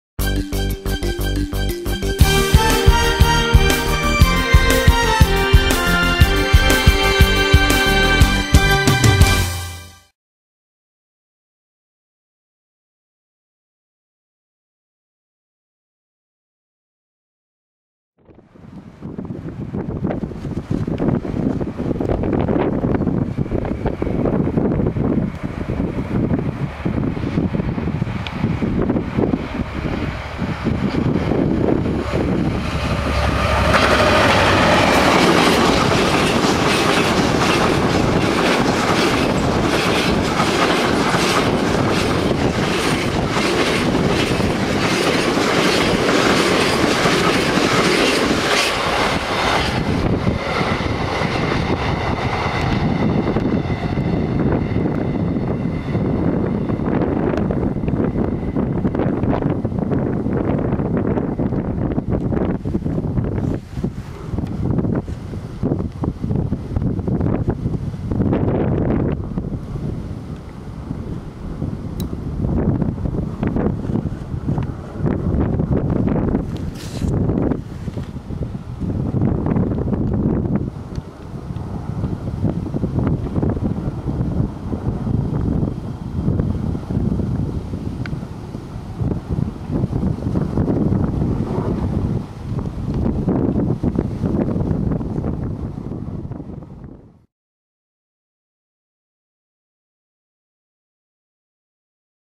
Bittern Line, Salhouse Station 17.12.2012
A single shot from the Bittern Line at Salhouse Station on the 17th December 2012.

GBRf Class 66, No. 66704 'Colchester Power Signalbox' is seen heading east towards North Walsham with the 6P41 Harwich Parkeston - North Walsham empty condensate tanks.

Filmed in the company of Chris (cmeonthemove) and Amy (AHHomeVideos).